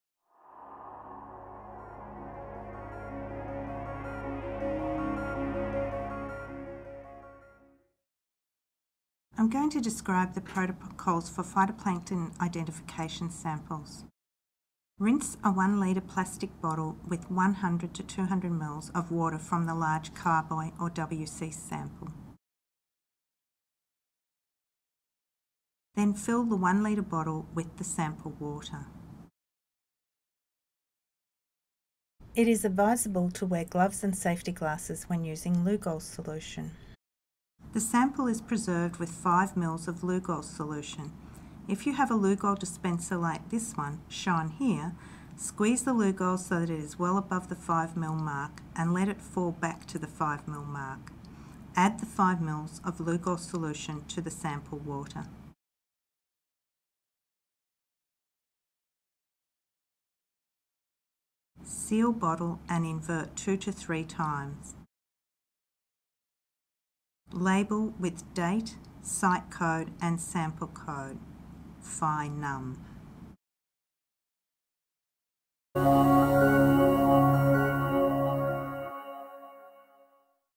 Protocol for IMOS microscopic phytoplankton sample collection
Instructional video for Integrated Marine Observing System (IMOS) biogeochemical water sampling procedures – Protocol for IMOS microscopic phytoplankton sample collection